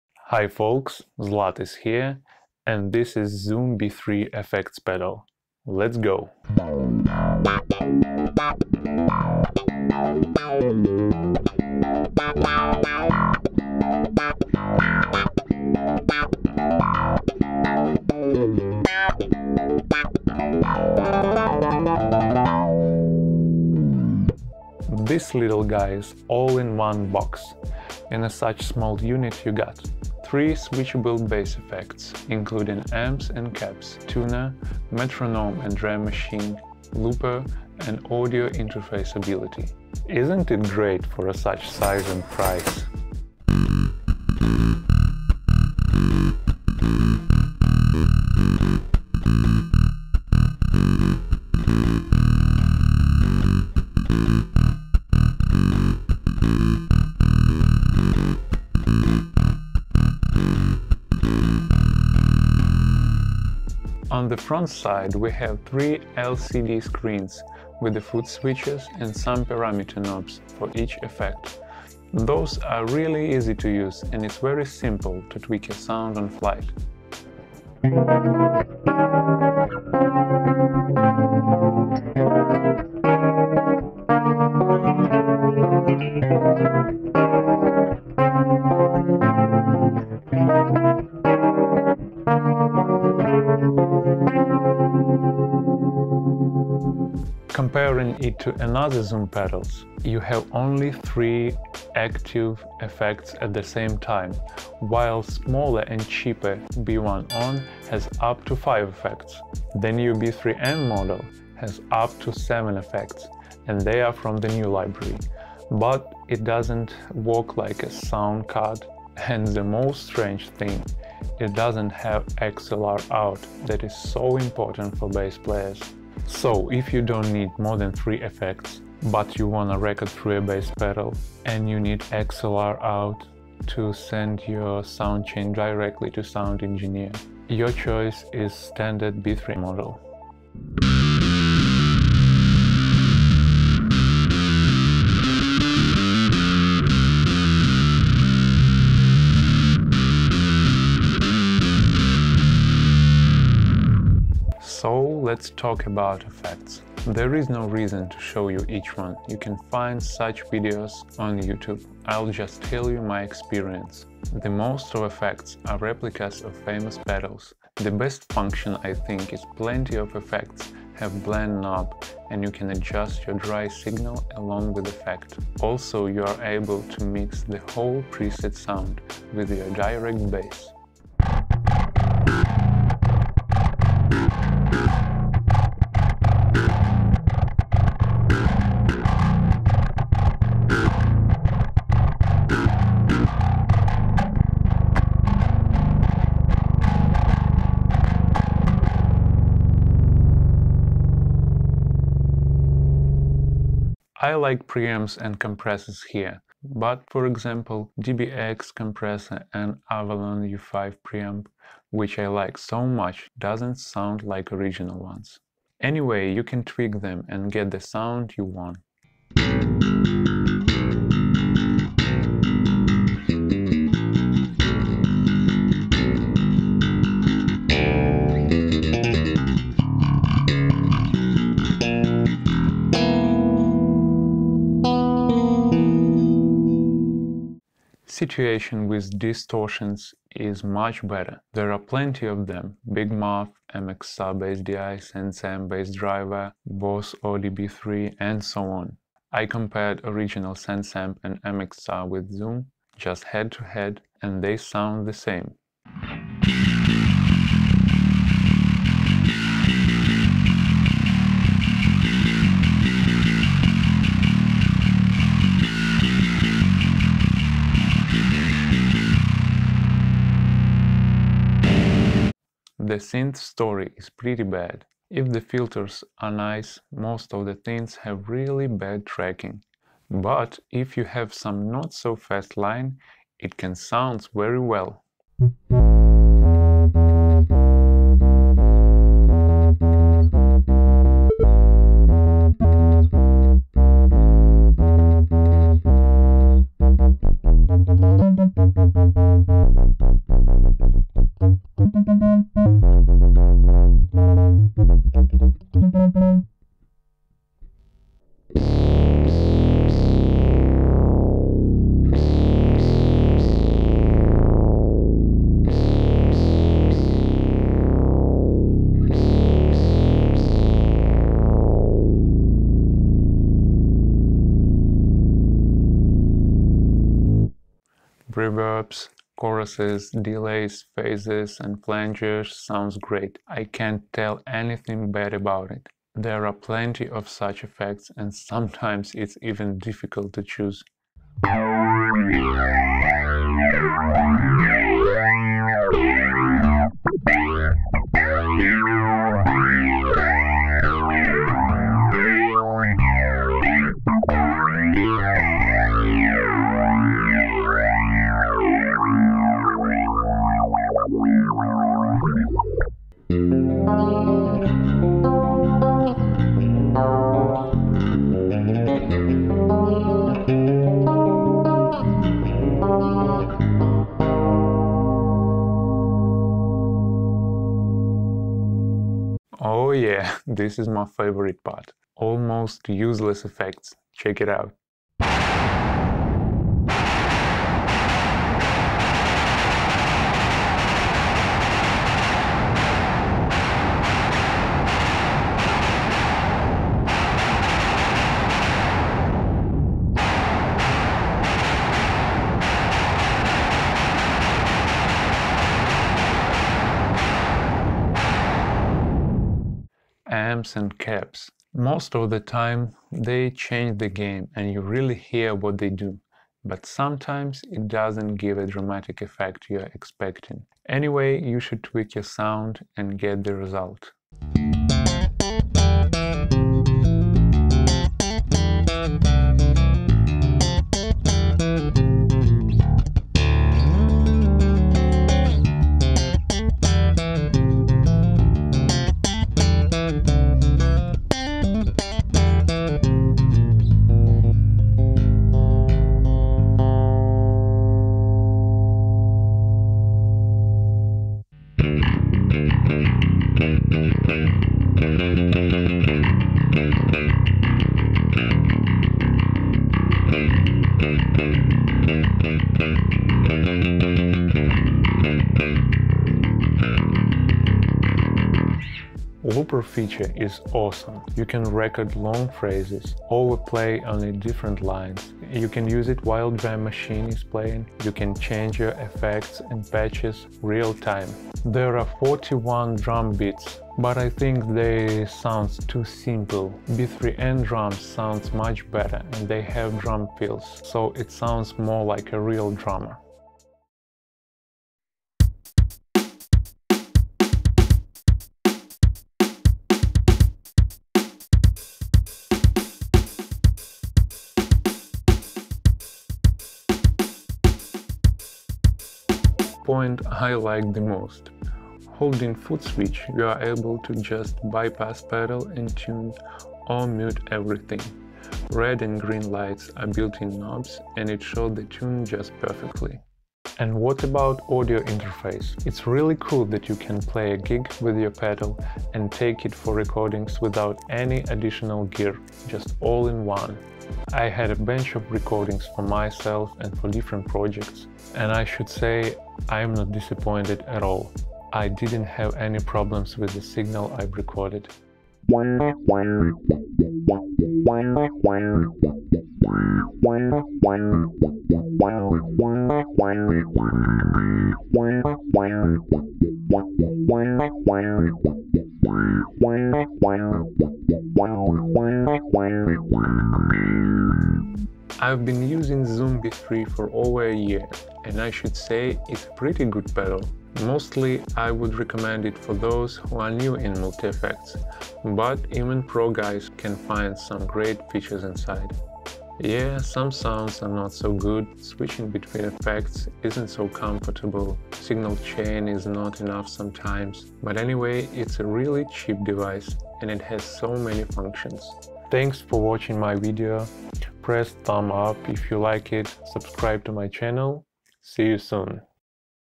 CHEAP and NICE - ZOOM B3 review
Hi guys, some of you asked me to make this video and here it is - review after 2 years of using. I've recorded video and instantly sold this pedal, hah.
You can watch/listen some examples of this box in mix on ma channel.

Sorry for my monotone voice and some exposition fails, I'm trying to do ma best.

Gear used: Warwick Thumb BO 6 bass with Bartolini pups, Zoom B3 pedal, MacOS system, Sony AS-300 and Nikon D3200 cams, FinalCutPro.